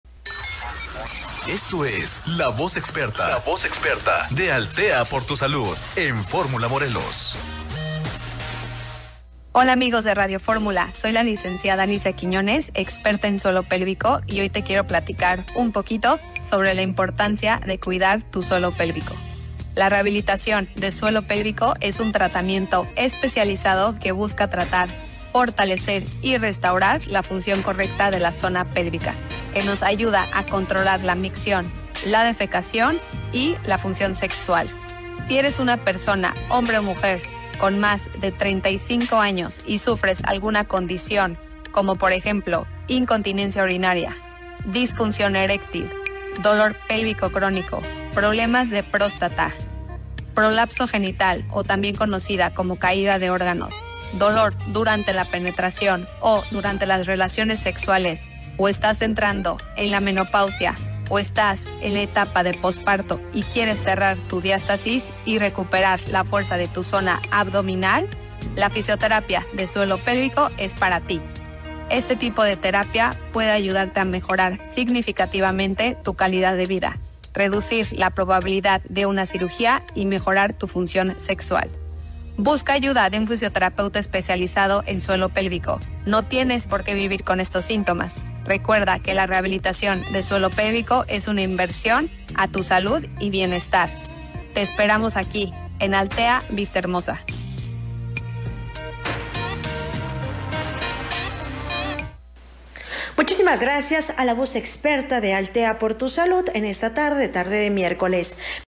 02/oct/2024 LaVozExperta de Althea por tu Salud, la Lic. en Fisioterapia Alicia Quiñones
El suelo pélvico y su importancia.